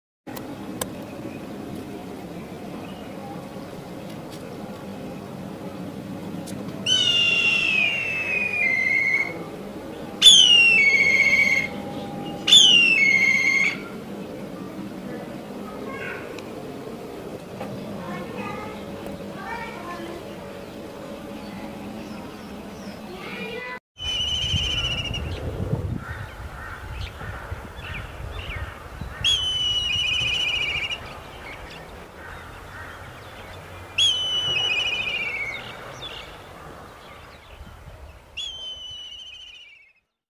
Black Kite call sounds in full hd voice / Black kite whistling in loud voice in kashmir
sounds of black kite with full hd voice in kashmir